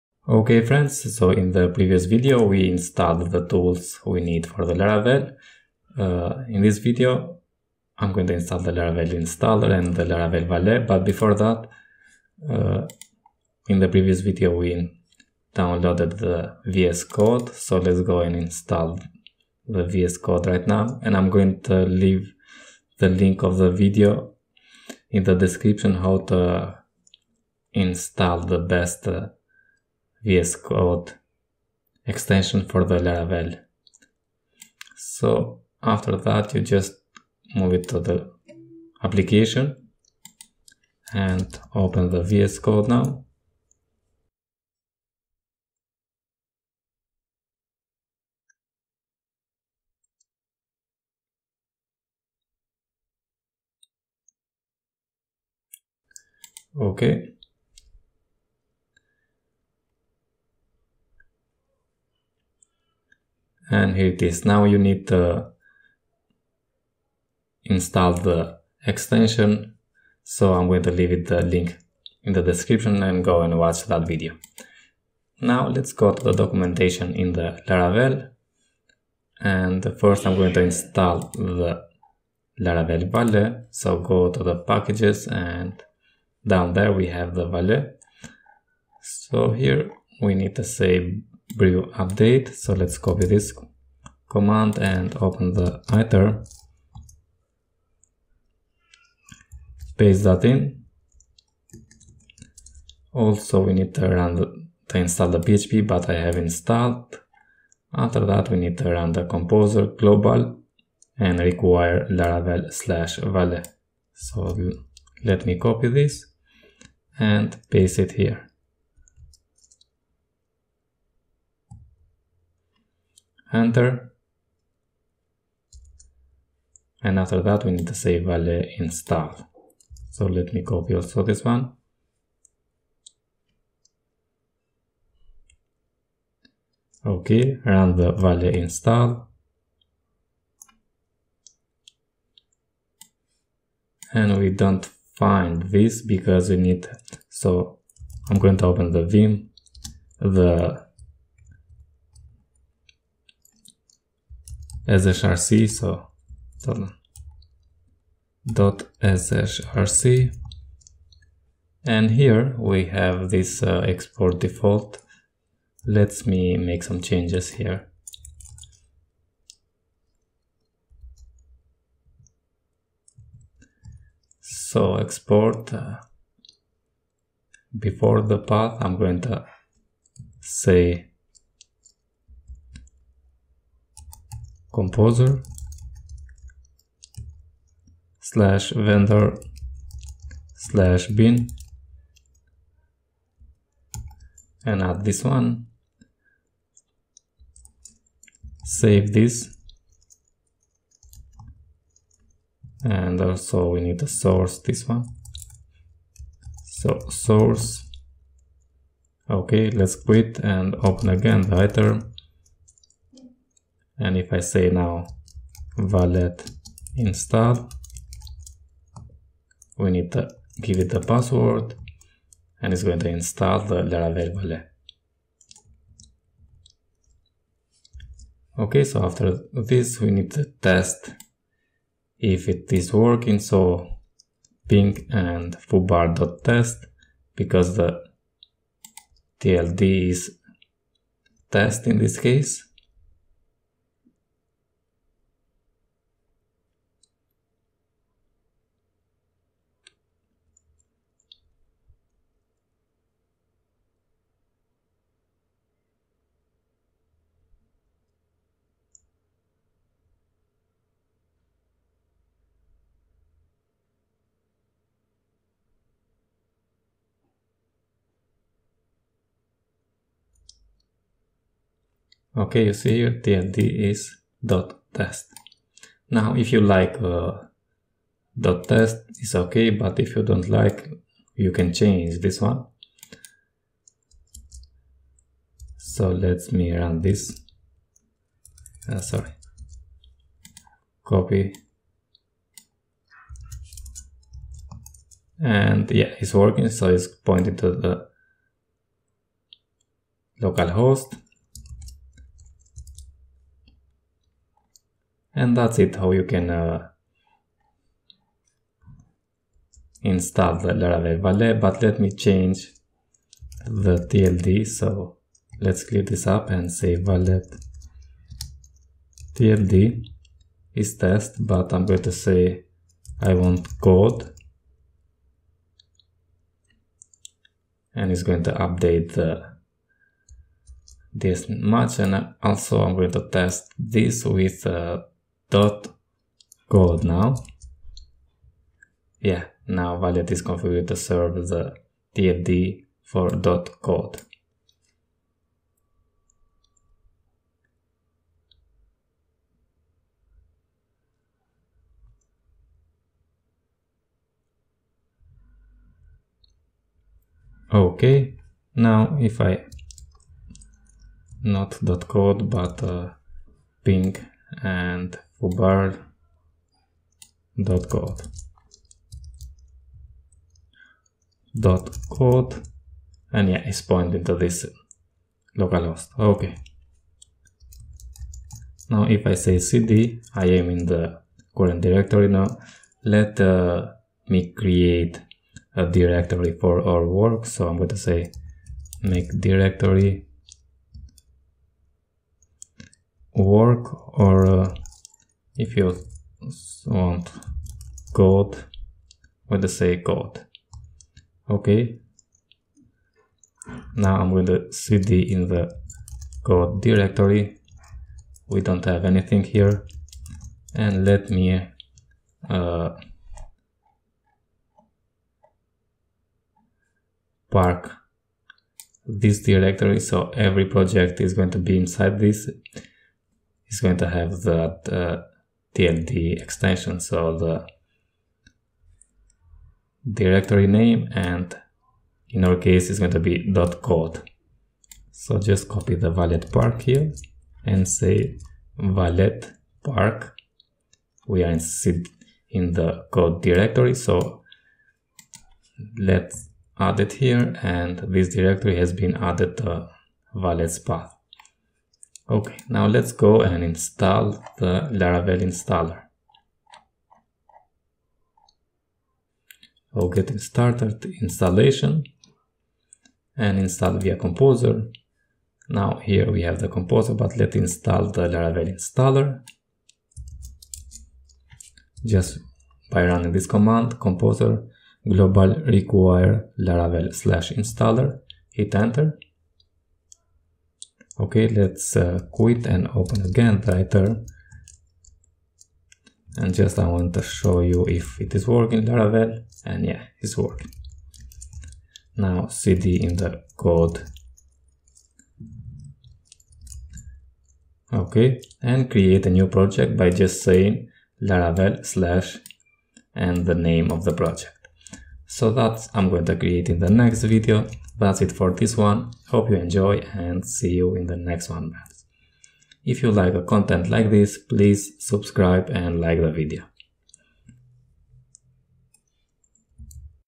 #2 Install Laravel 9 on Mac | Laravel 9 Essentials Tutorial
Install Laravel valet on mac, and Laravel installer.
New series about Laravel. Laravel essentials 101.
Tools we need for working with Laravel.
Laravel setup.
Laravel 9 Tutorial.
Laravel tutorial.